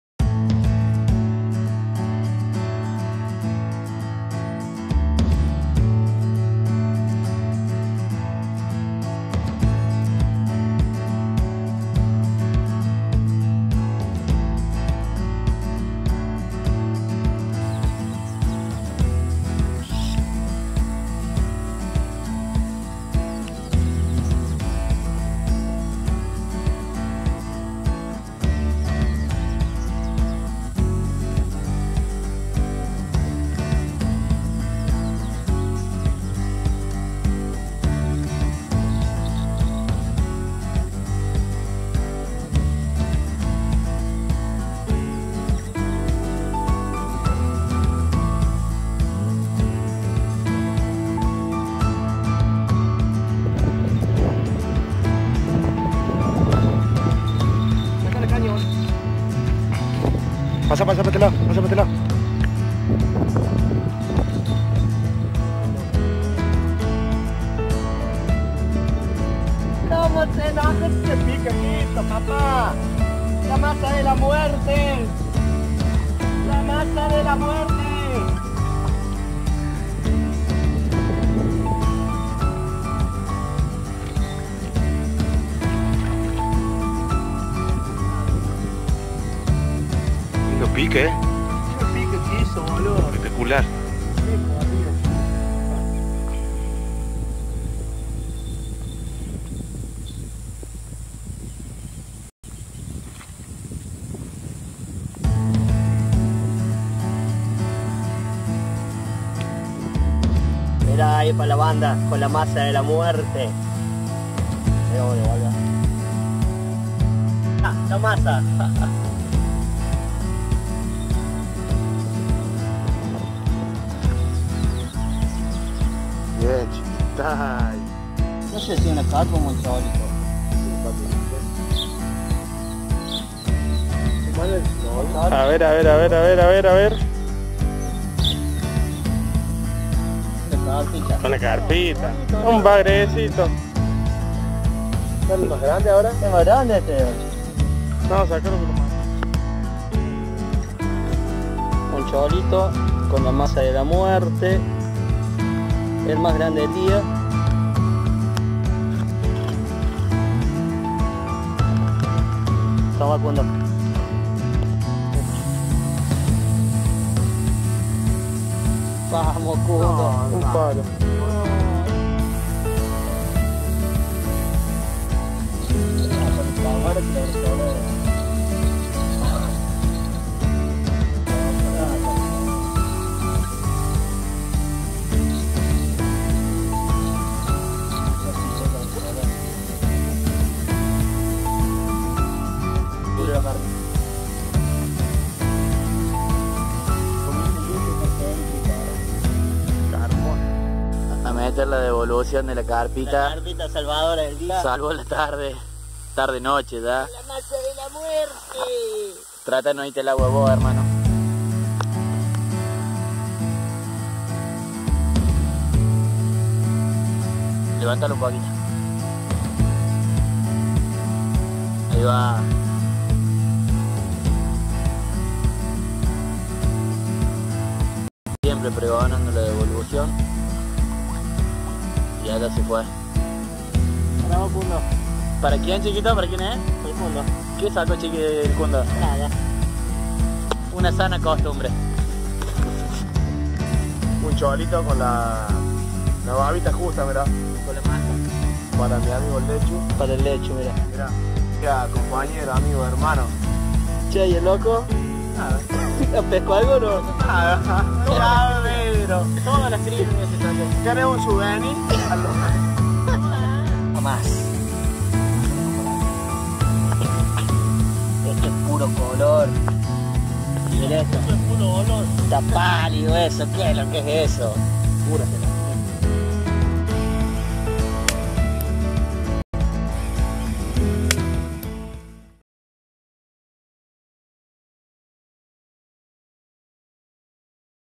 💯COMO PESCAR CARPAS Y BAGRES Con MASA⁉️
tutorial de como pescar  bagres y CARPAS con masa, la pesca se realizó en canales del interior de Córdoba, argentina. El preparado de la masa lo estaremos subiendo en el próximo vídeo. Saludos y buenas pescas!!